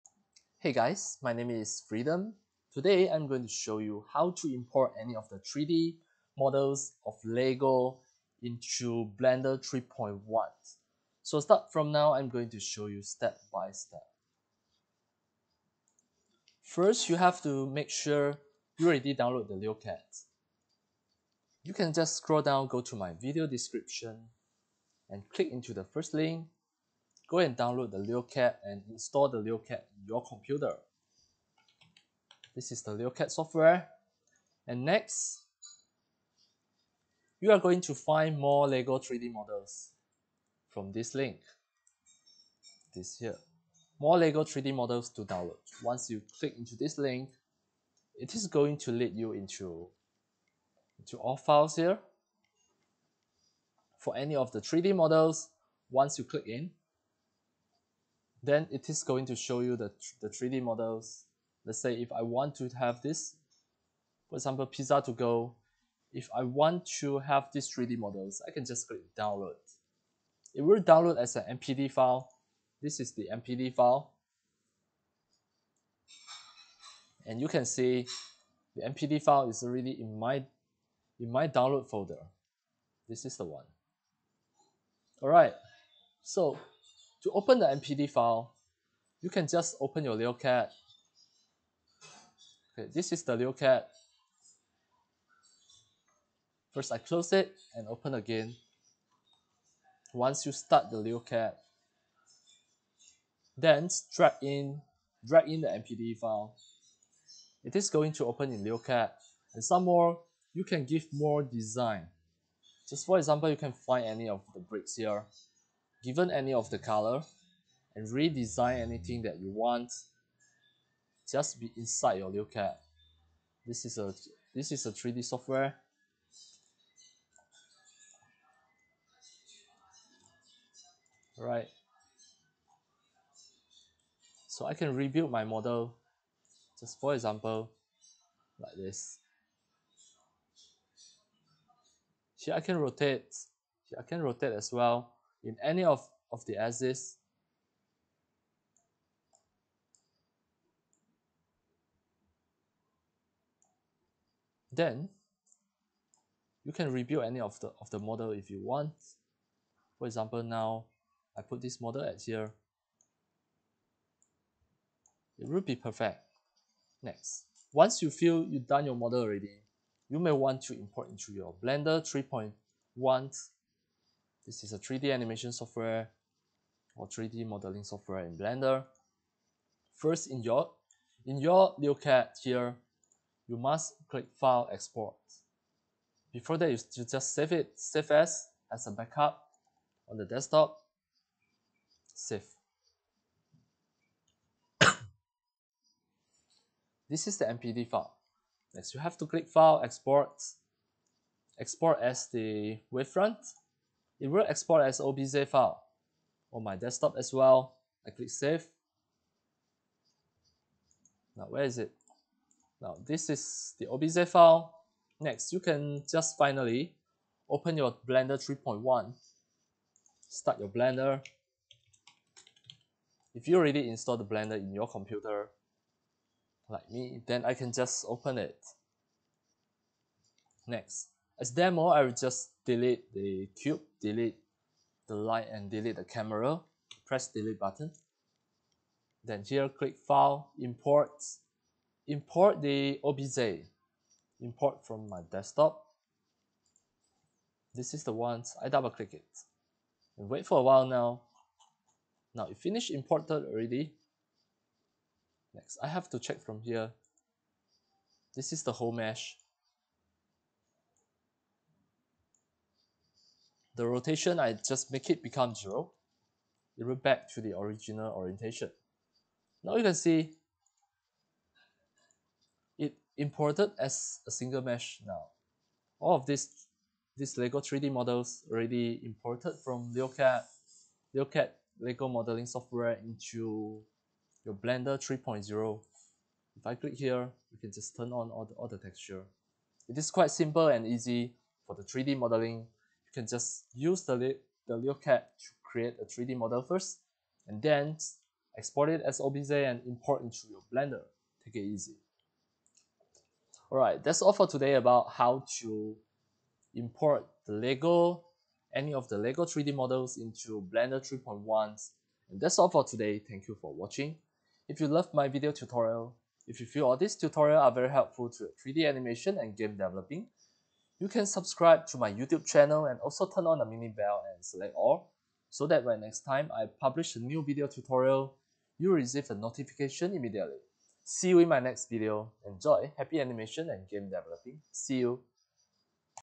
LEGO to Blender 3.1 - Full Tutorial - LeoCAD
- Hey guys, how to create any LEGO 3D models and import into Blender 3.1? This is my step-by-step tutorial on how to do this! ENJOY! and have fun! Happy animation and game dev! ENJOY!

Your friend,
FREEDOM

Get iClone and CC3 Contents